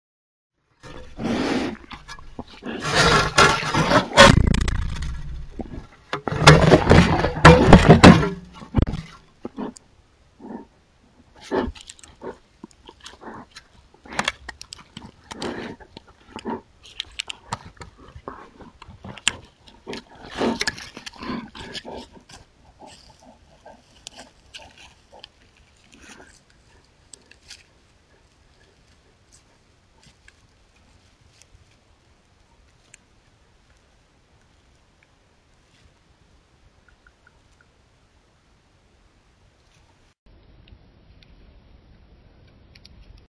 Black Bear Attacking and Then Walking Away from Our Trail camera (Jan 29, 2024)
This was filmed in January 2024; the cold weather damaged the Time, Date, and Temp stamp on this camera. No, it was not filmed in 1971.

From Wiki: The American black bear (Ursus americanus), also known as the black bear, is a species of medium-sized bear endemic to North America. It is the continent's smallest and most widely distributed bear species. The American black bear is an omnivore, with a diet varying greatly depending on season and location. It typically lives in largely forested areas, but will leave forests in search of food, and is sometimes attracted to human communities due to the immediate availability of food.